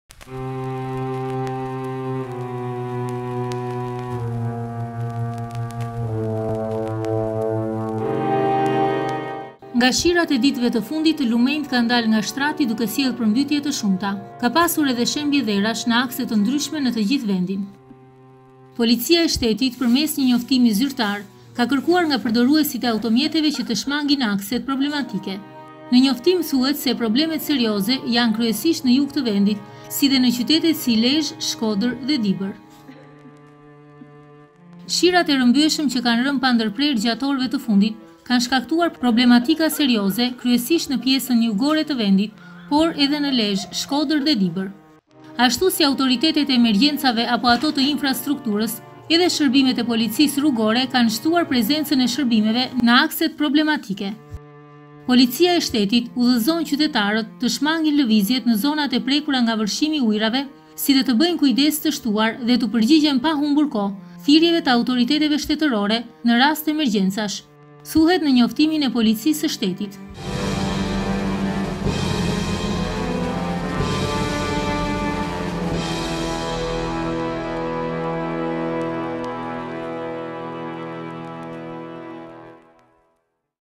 Përmbytjet, Policia e Shtetit paralajmërim qytetarëve për akset më të rrezikuara
Nga shirat e ditëve të fundit lumenjtë kanë dalë nga shtrati, duke sjellë përmbytje të shumta. Ka pasur edhe shembje dherash në akse të ndryshme në të gjithë vendin.